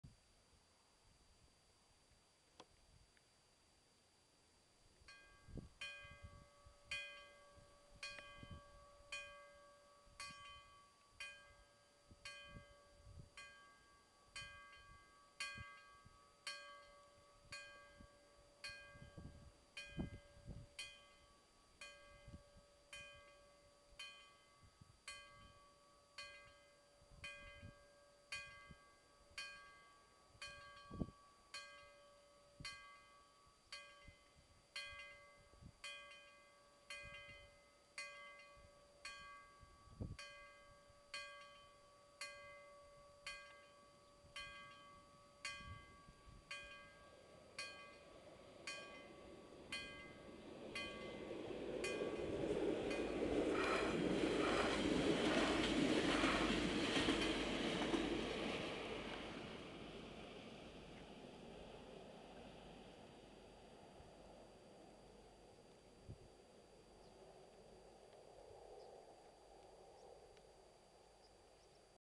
Danish IC3 train at railroad crossing
A Danish regional train IC3 (class MF 5013) going appr. about 75 km/h (46,5 mph) over a railroad crossing at Stavreby in Denmark, on 4th Oct 2008.